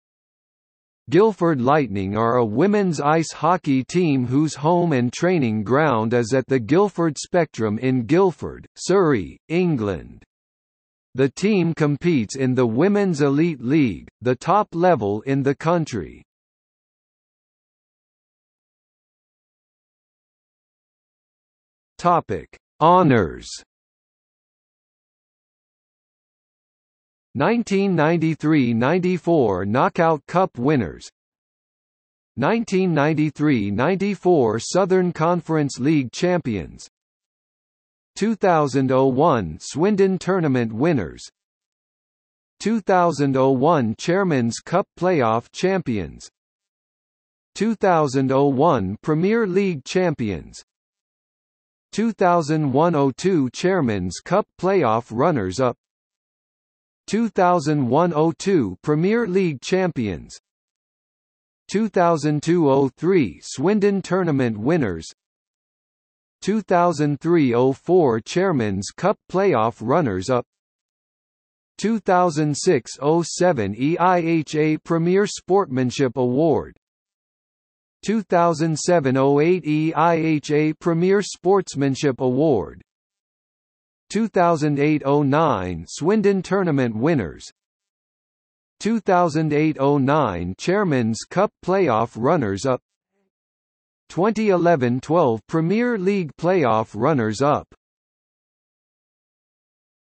Guildford Lightning | Wikipedia audio article
00:00:18 Honours

- Socrates

SUMMARY
Guildford Lightning are a women's ice hockey team whose home and training ground is at the Guildford Spectrum in Guildford, Surrey, England. The team competes in the Women's Elite League, the top level in the country.